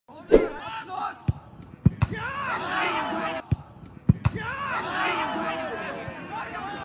Der Verteidiger des @fc_moosburg klärt hier Souverän 🫨👀🎥 Danke an Stephan Schikowski
FuPa präsentiert dir die besten Tore und Highlights des Amateurfußballs. ✔